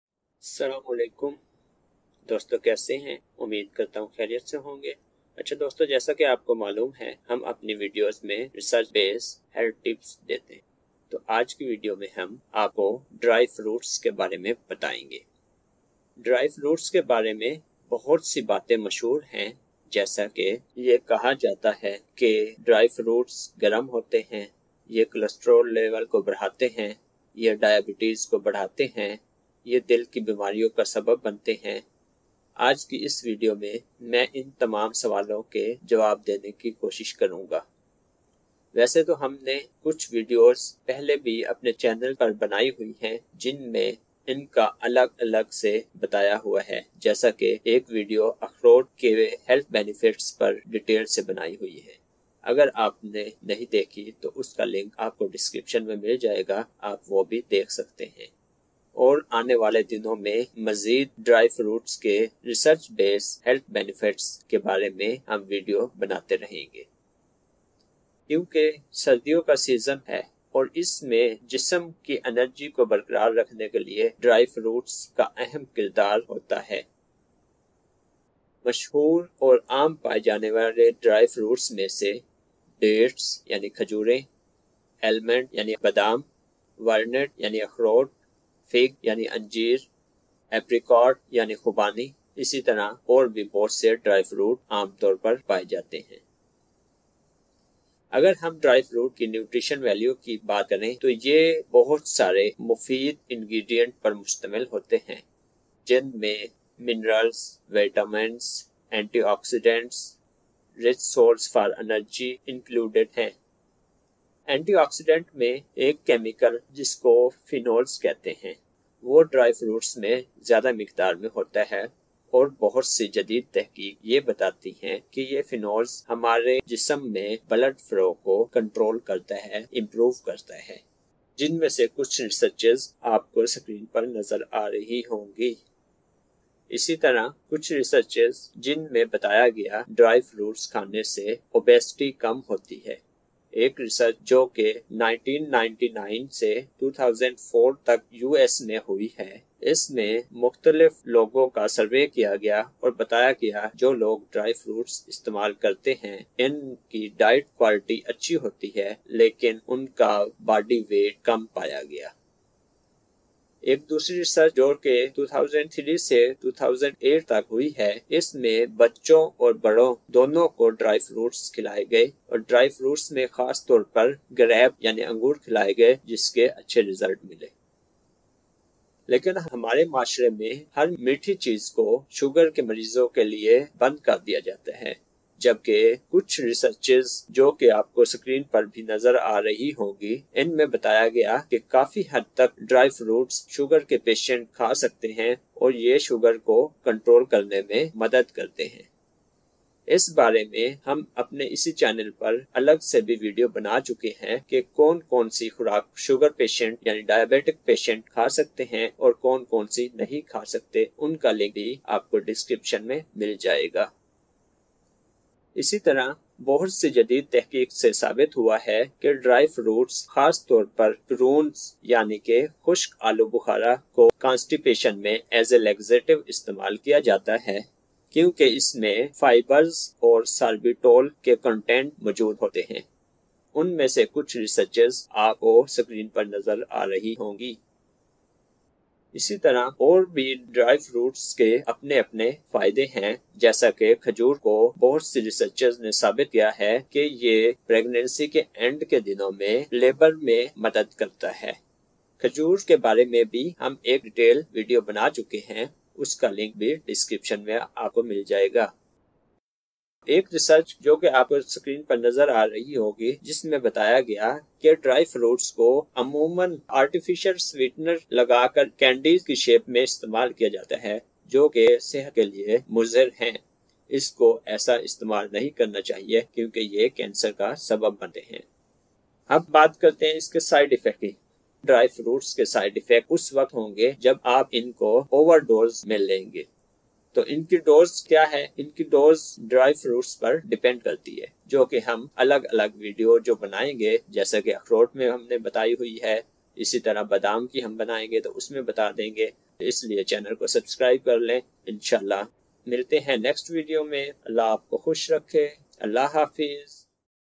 Dry Fruits: The Science Behind the Health Benefits of Dry Fruits | Dry Fruits Ke Fayde
In this video, Unlock the amazing health benefits of Dry Fruits! In this video, Dr. Mudasir Saeed delves deep into the research, revealing surprising ways Dry Fruits can improve your health. Learn about their benefits for digestion, immunity, diabetes management, and more! Nature's Secret to Health and Wellness, Comprehensive Dry Fruits Benefits / Dry Fruits Ke Fayde.

اس ویڈیو میں ڈاکٹر مدثر سعید تحقیق پر مبنی خشک میوہ جات کے صحت کے فوائد بتا رہے ہیں۔ دل کی صحت کو بہتر بنانے سے لے کر دماغی افعال کو بڑھانے تک، خشک میوہ جات فوائد کے پیچھے سائنس کے بارے میں مزید جاننے کے لیے یہ ویڈیو دیکھیں اور آپ خشک میوہ جات کو اپنی خوراک میں کیسے شامل کر سکتے ہیں! مزید صحت اور تندرستی کی بصیرت پر اپ ڈیٹ رہنے کے لیے لائک، شیئر، اور سبسکرائب کرنا نہ بھولیں۔!